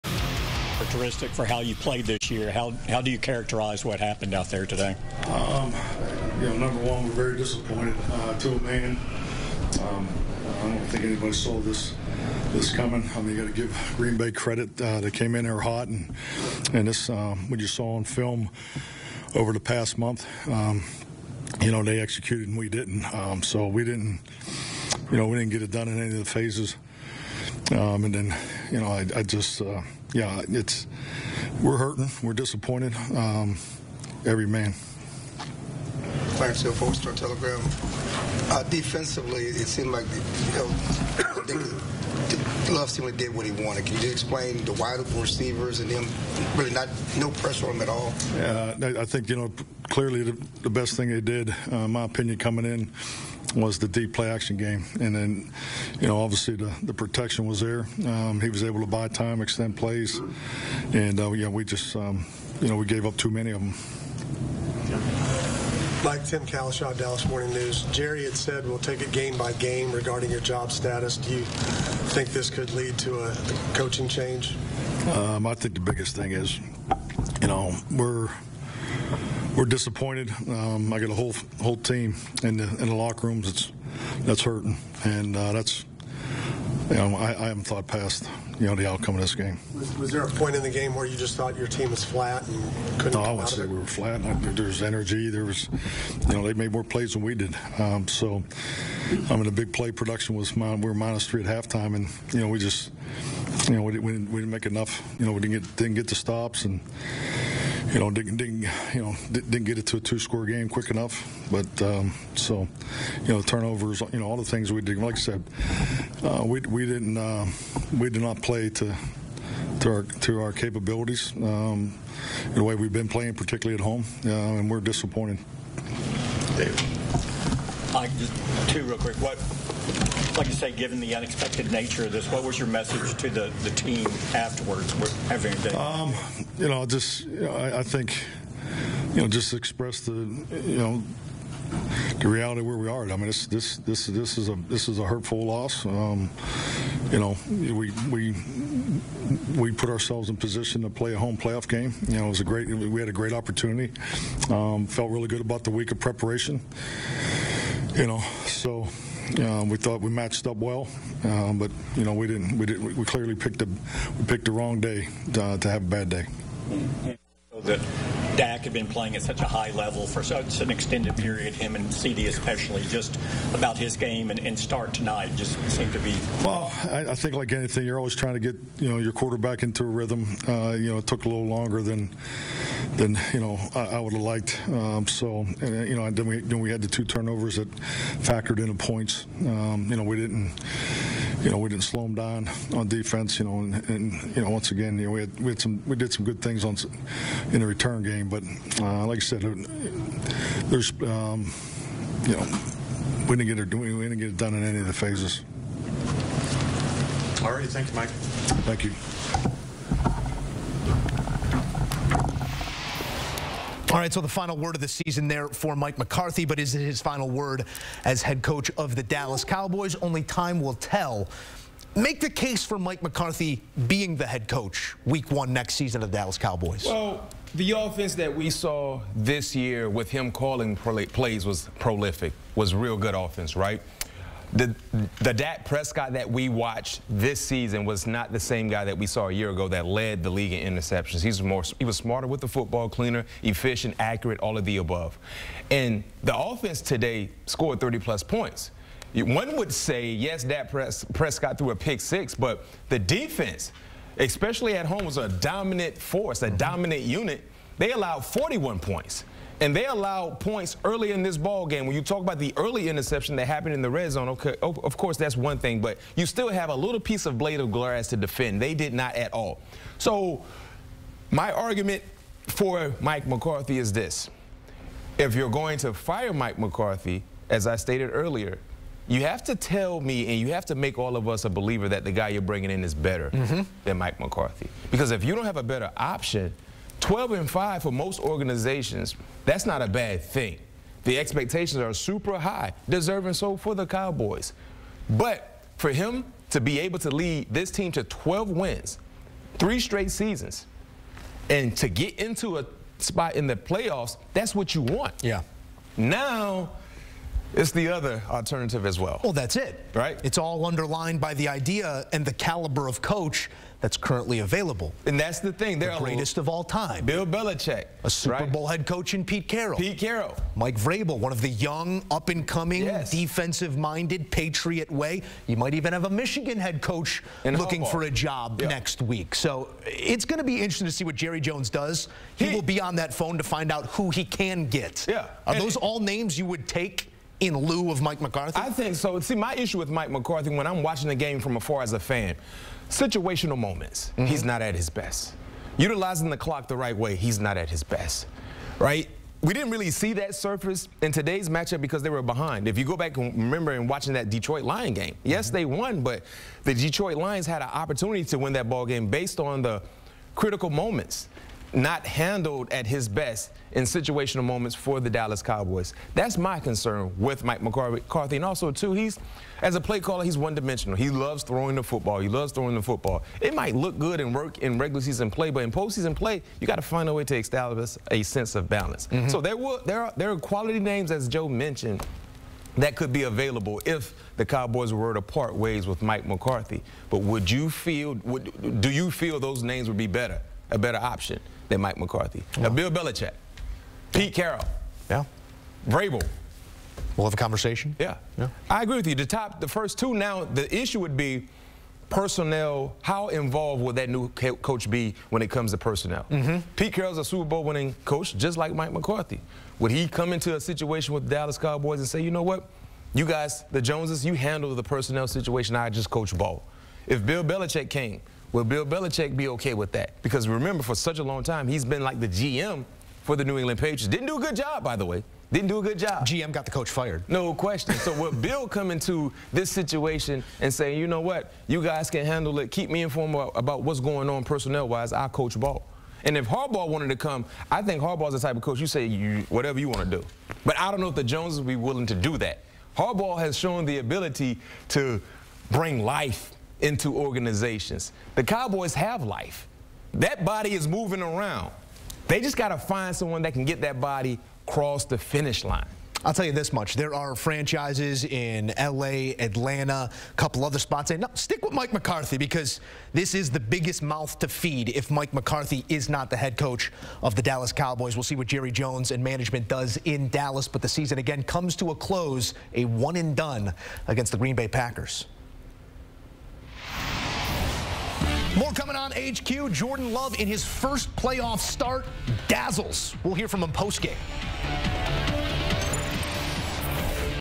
Mike McCarthy says Dallas "DIDN'T PLAY TO CAPABILITIES" in LOSS to Packers | CBS Sports
Mike McCarthy sounds off following Dallas' loss to Green Bay Packers.